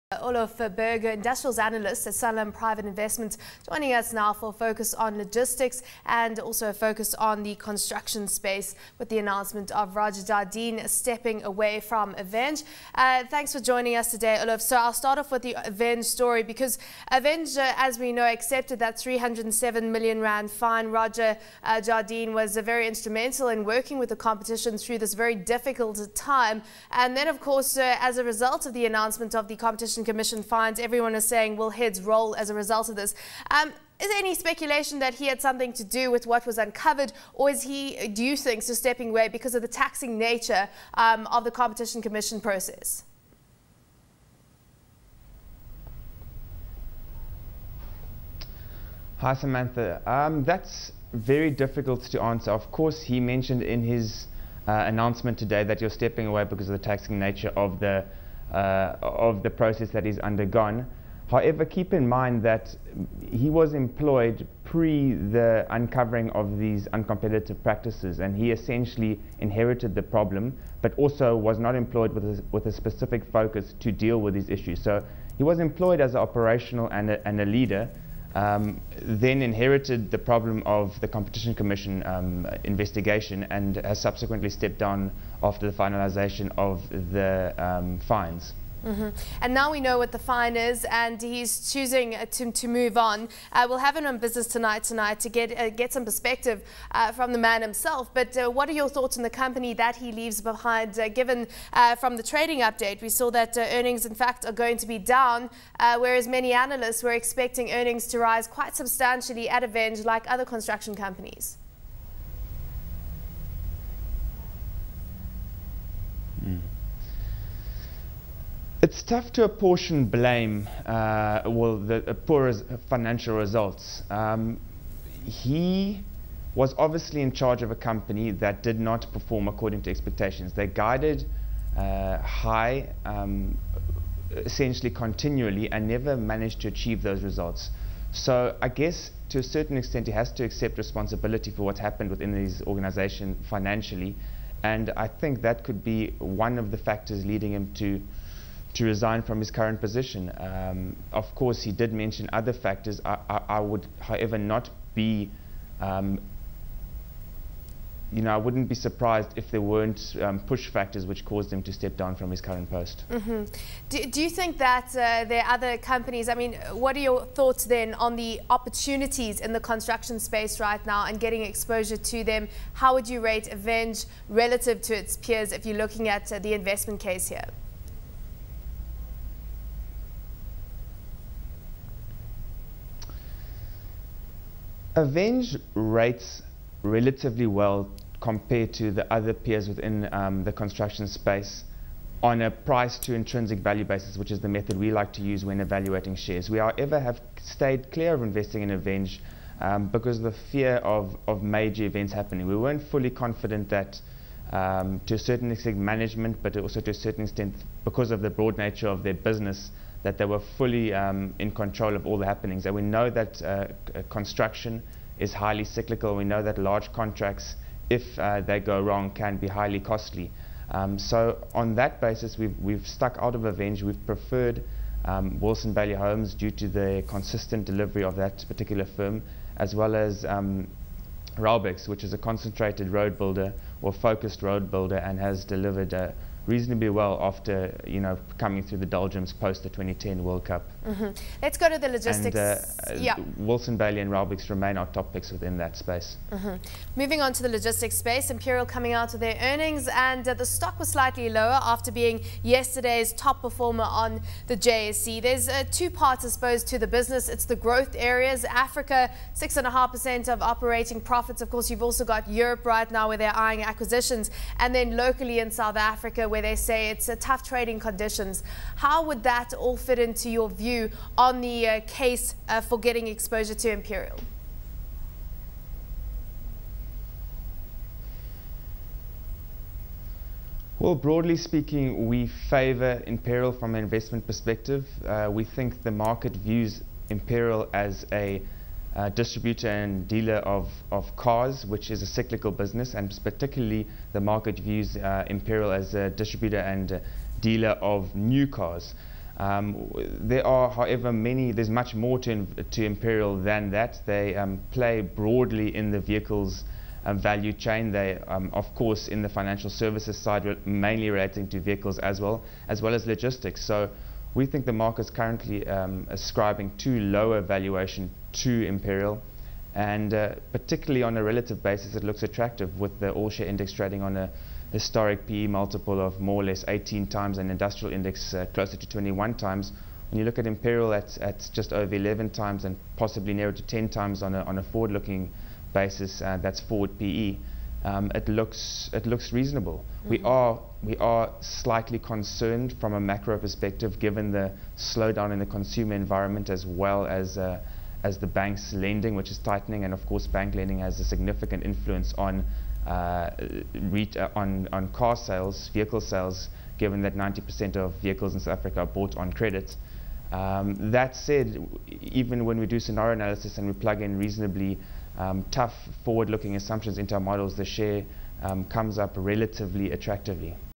The state of logistics and beverages sector
Olof Bergh Industrials Analyst at Sanlam Private Investments Joins ABN for a focus on the logistics and beverages sector with reporting companies Distell and Imperial at the focal point.